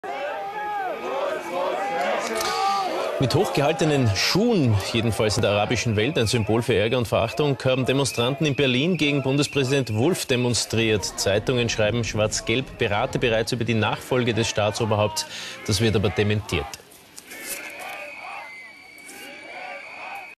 Wenn Deutsche "Arrabbiati" meinen sie wären Araber...
ca. 300 Deutsche demonstrieren auf arabisch.
Es gilt als gänzlich ausgeschlossen, daß die BILD 400 Schuhverkäufer für diese DEMO engagiert hat.